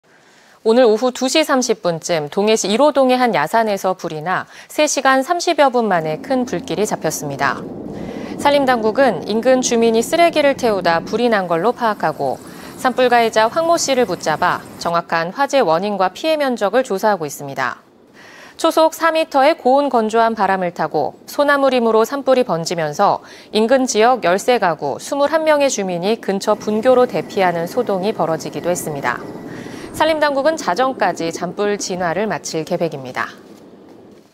[단신] 동해시 이로동 산불 3시간 30분 만에 주불 진화 완료 20220222
오늘(22일) 오후 2시 30분쯤
동해시 이로동의 한 야산에서
산불이 나,
3시간 30여 분 만에
큰 불길이 잡혔습니다.

산림당국은 인근 주민이 쓰레기를 태우다
불이 난 걸로 파악하고,
산불 가해자 황모 씨를 붙잡아
정확한 화재 원인과 피해 면적을

초속 4m의 고온 건조한 바람을 타고
소나무림으로 산불이 번지면서,
인근 지역 13가구, 21명의 주민이
근처 분교로 대피하는
소동이 벌어지기도 했습니다.

산림당국은 자정까지
잔불 진화를 마칠 계획입니다.